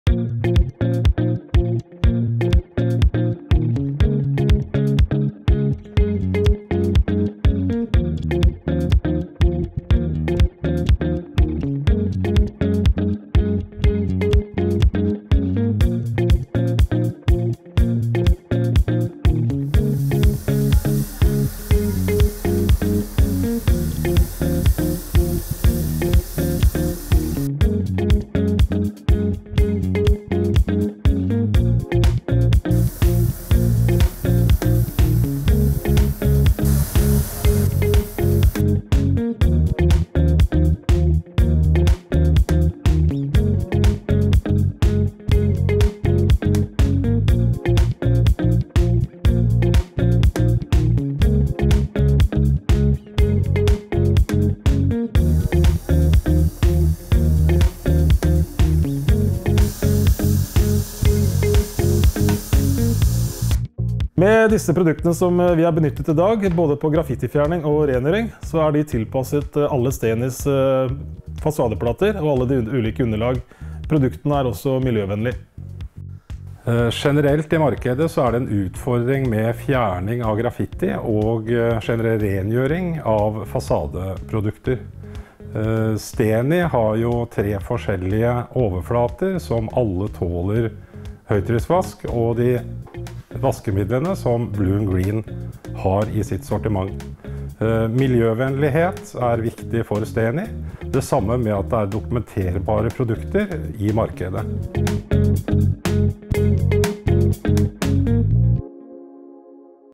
Fjern graffiti riktig
Graffiti kan føre til tidkrevende rengjøring og frustrasjon. Her viser vi deg hvordan STENIs fasadeplater blir helt rene og fine når vaskeprosessen gjøres riktig.